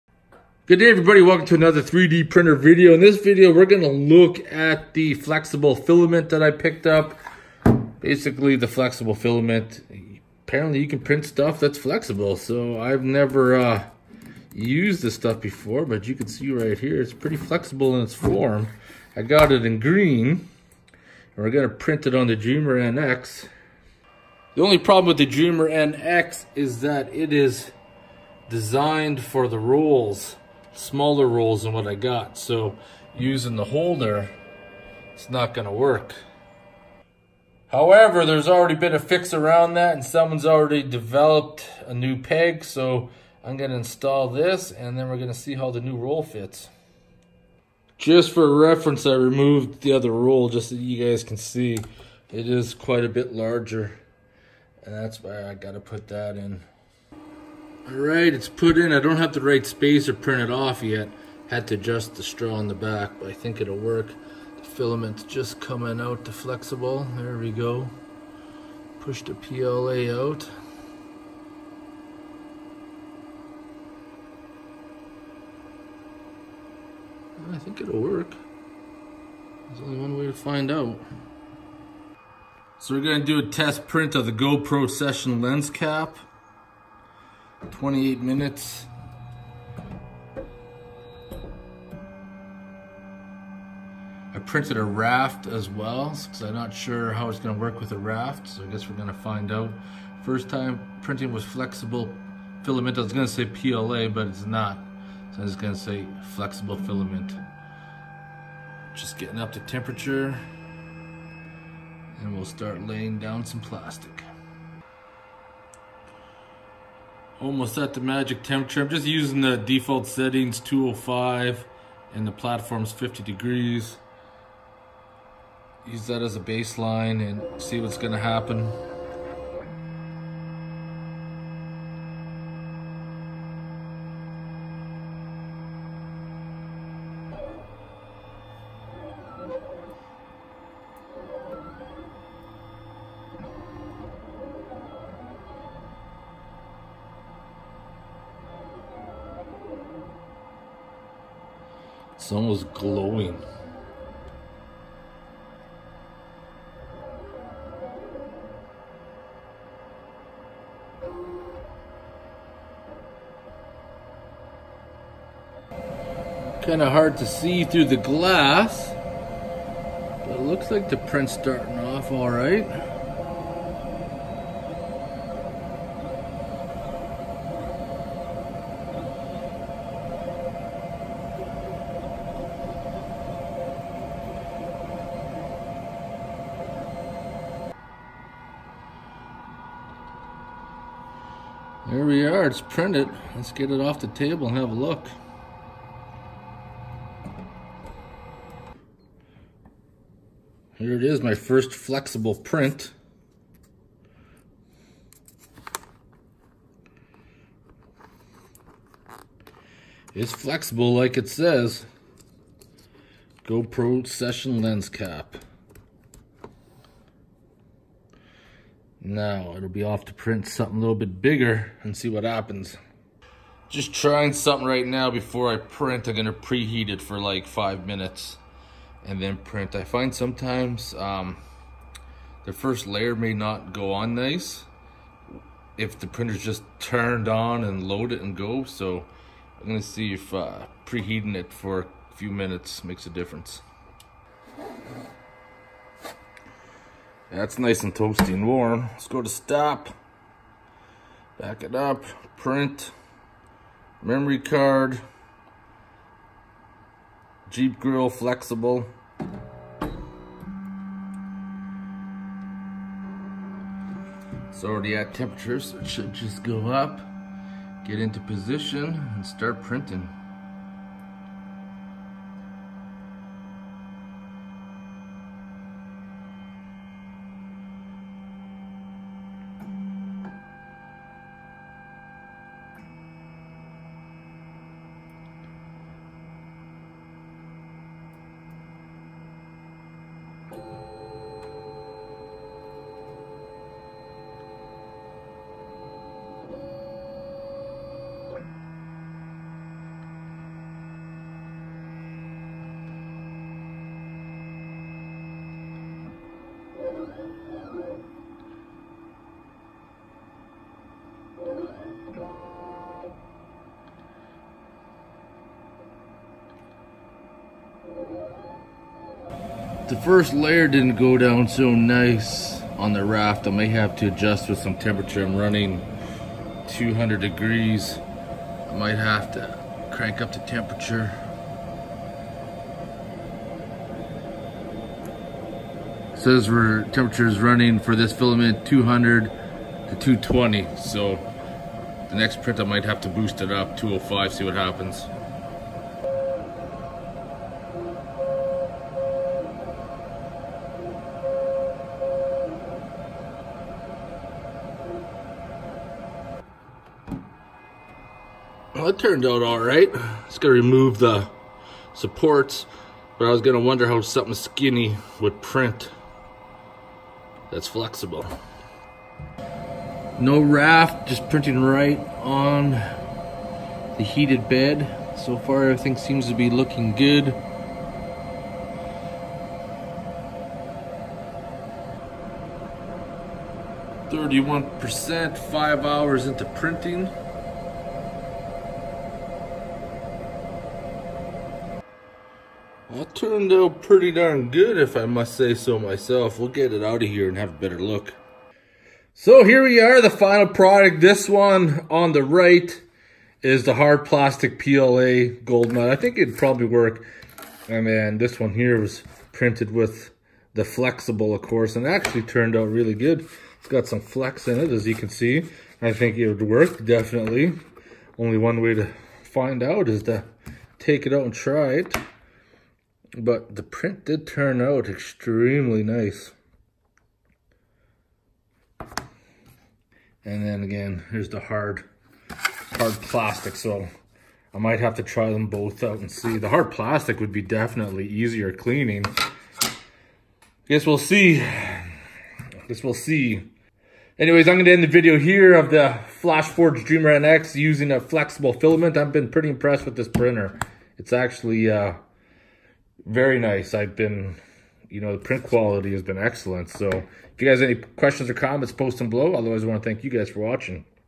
FlashForge Dreamer NX Flexible Filament test
Todays video i'm trying something new with the Flashforge dream Nx 3d printer.  i've been using pla filament for quite sometime now and wanted to try something different so i got myself some flexible filament to try out.  Flexible filament has many uses as i'm wanting to print some gold matting to use for the upcoming prospecting season.